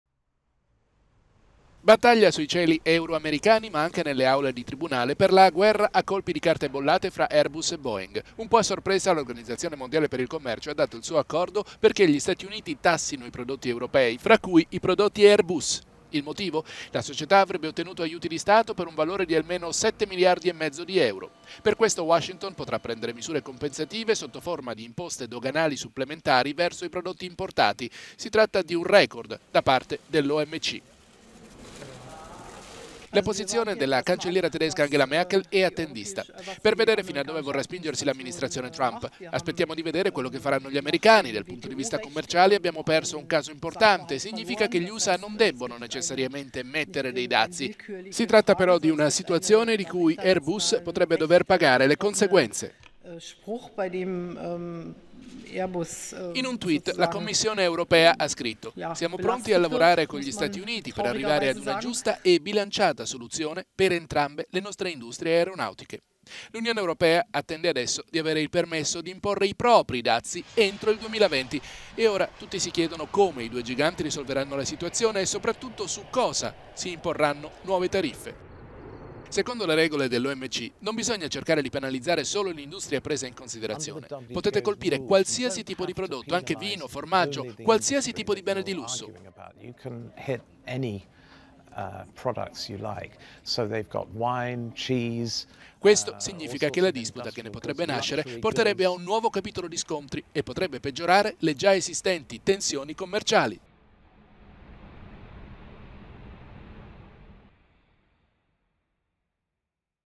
Caso Airbus-Boeing, Europa condannata
L'Unione Europea avrebba fornito aiuti pubblici alla compagnia. L'Omc sanziona Bruxelles…